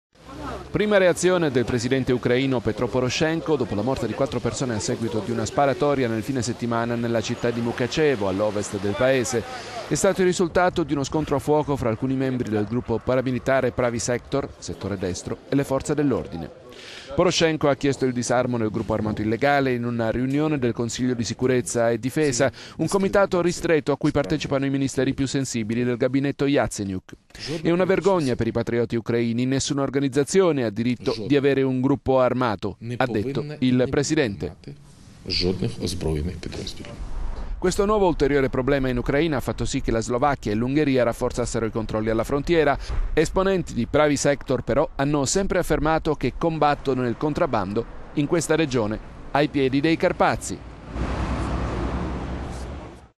Ucraina, Poroshenko: "nessun gruppo paramilitare nel nostro paese"
Prima reazione del presidente ucraino Petro poroshenko dopo la morte di quattro persone a seguito di una sparatoria nel fine settimana nella città di Mukachevo, all'ovest del paese.

È stato il risultato di uno scontro a fuoco fra alcuni membri del gruppo paramilitare Pravy Sektor, settore destro, e le forze dell'ordine.

Poroshenko ha chiesto il disarmo del gruppo armato illegale in una riunione del consiglio di sicurezza e difesa, un comitato ristretto a cui partecipano i ministeri più sensi…